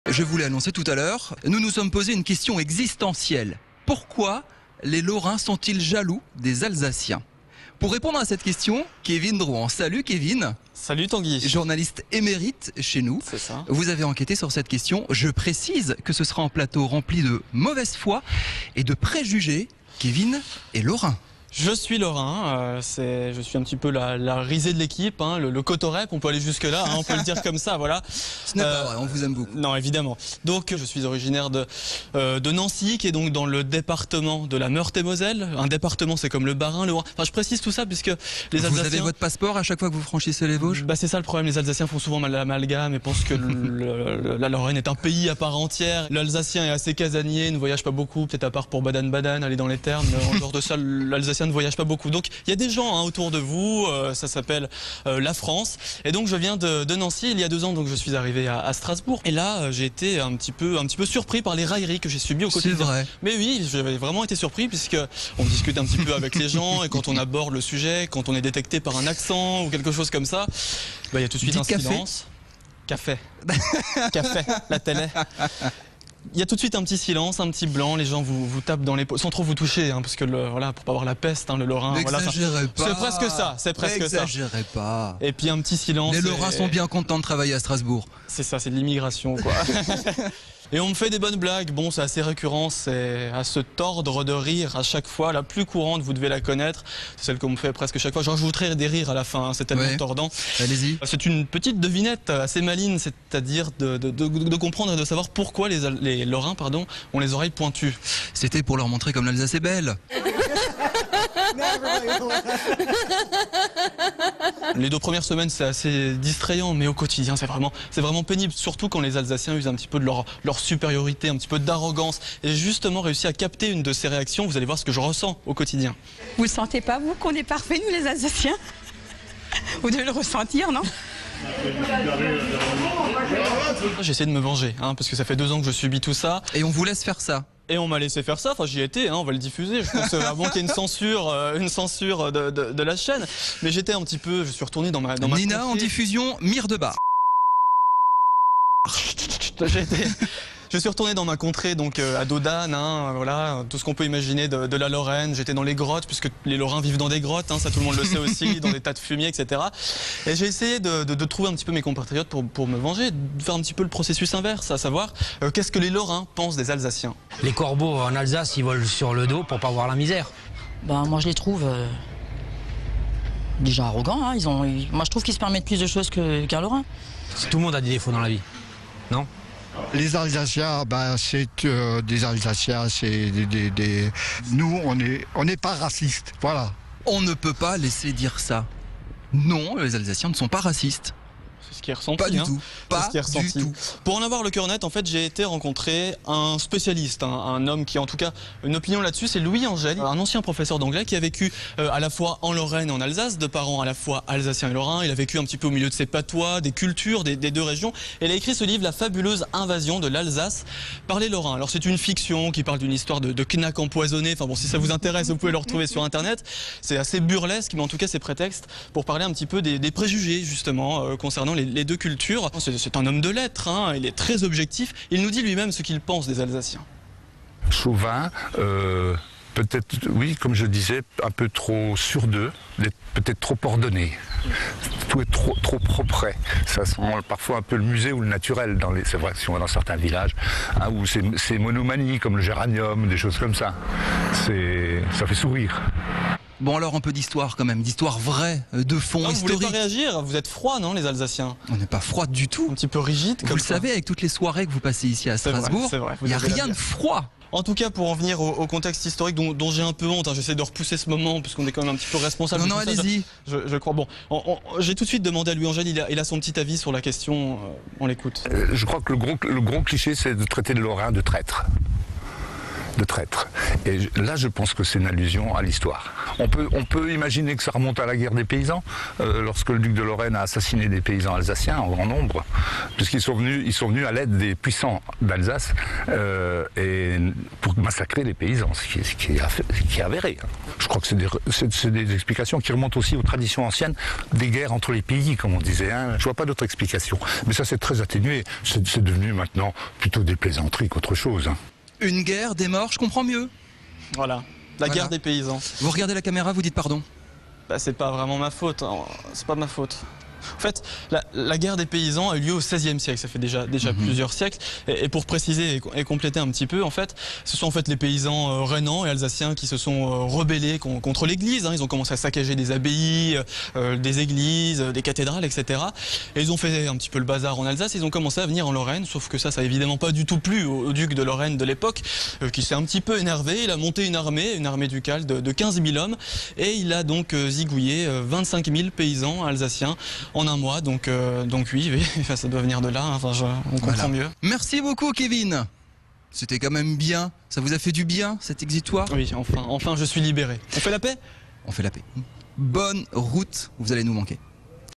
Pourquoi les Lorrains sont-ils jaloux des Alsaciens?
Sites webs/blogs, copiez cette vidéo avec le bouton "Intégrer" !
 Débat entre Tanguy Flores, rédacteur en chef d'Alsace 20, et Kevin Drouant, journaliste lorrain pour la chaîne Alsace 20, sur la question : pourquoi les Lorrains sont-ils jaloux des Alsaciens ?